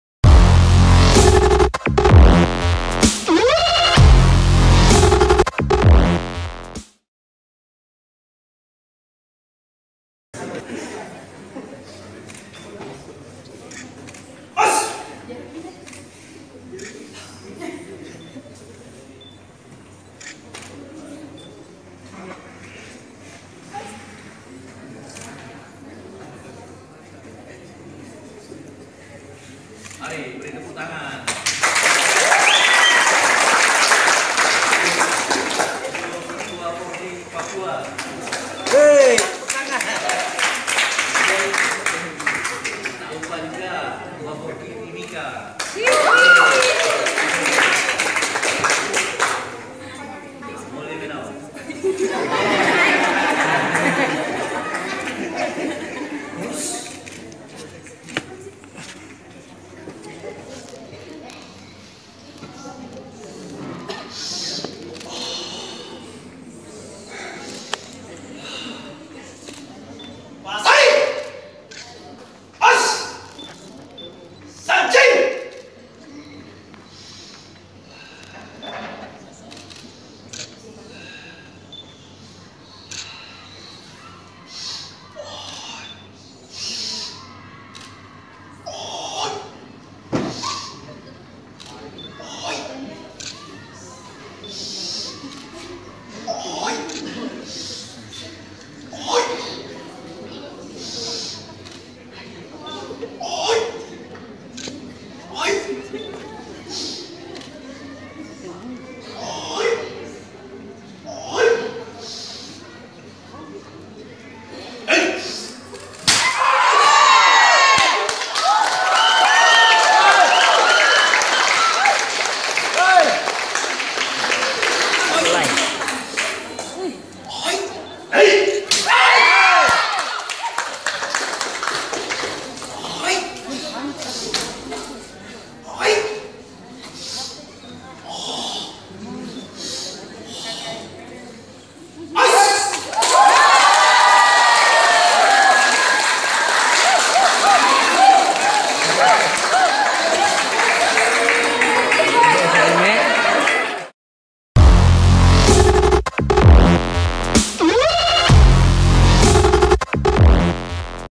DEMO SANCHIN KUSHIN RYU
Peragaan KATA Sanchin dalam kegiatan pelantikan PENGURUS FORKI KABUPATEN MIMIKA PERIODE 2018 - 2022

Karateka :
Decky. L Mirino

KUSHIN RYU M KARATE-DO
KKI MIMIKA